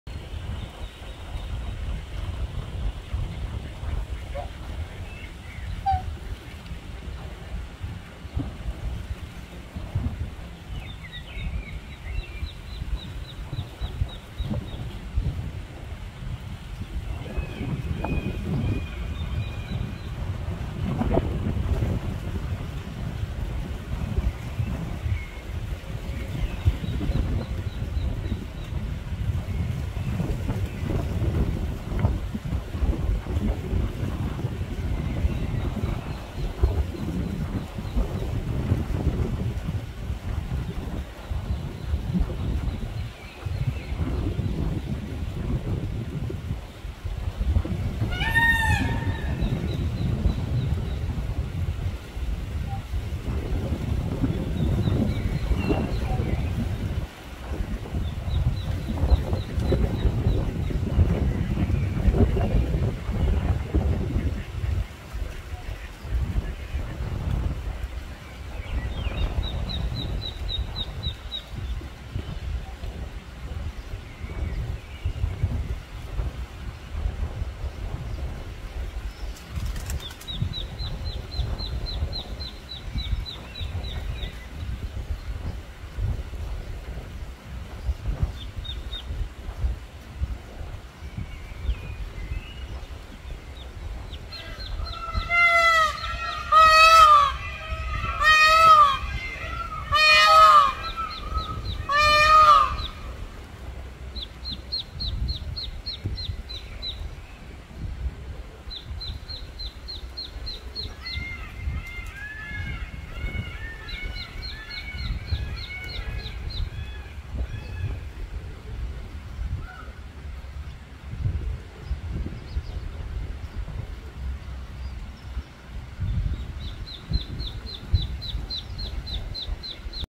Relaxation and sleep with birdsong.- Sound of birds singing, leaves rustling and water noise.
Bird sounds for good mood, relaxation, sleep.